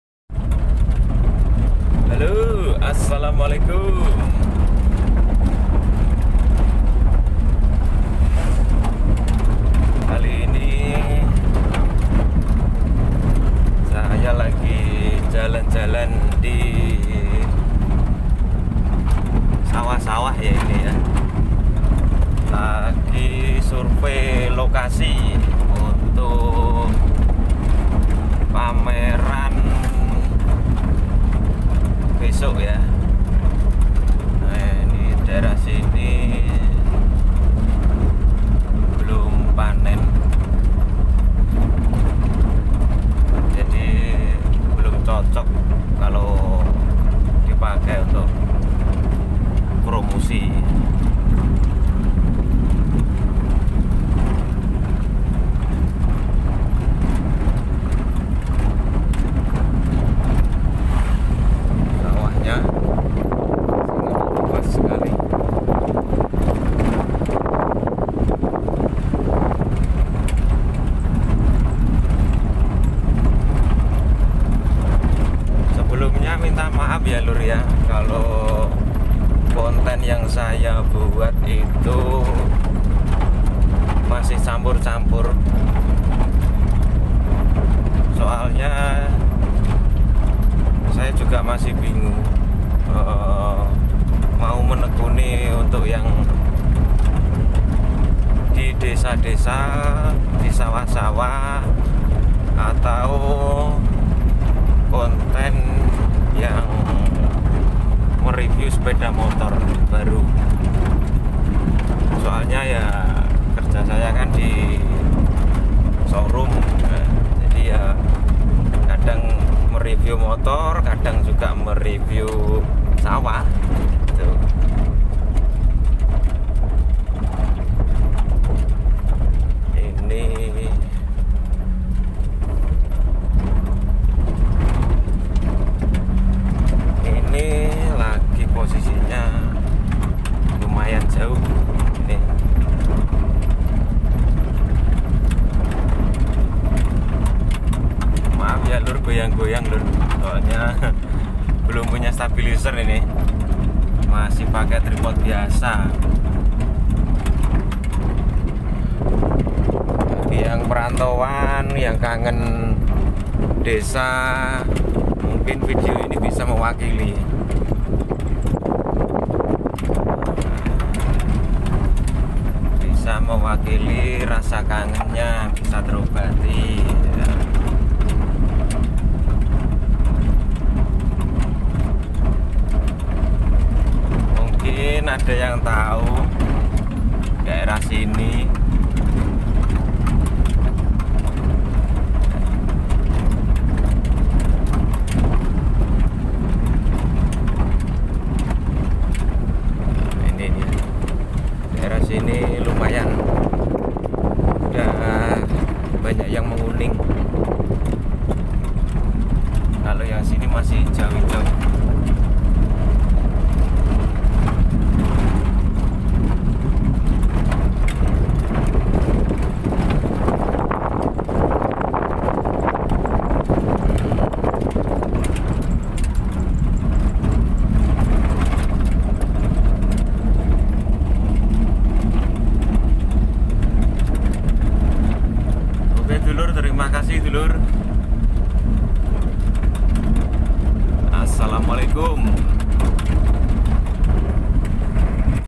Penampakan persawahan
Assalamualaikum,, kali ini kita lagi liat liat pemandangan sawah sekalian surve lokasi untuk pameran motor,, mohon maaf sedulur apabila video yang saya buat masih campur campur .. 🙏🙏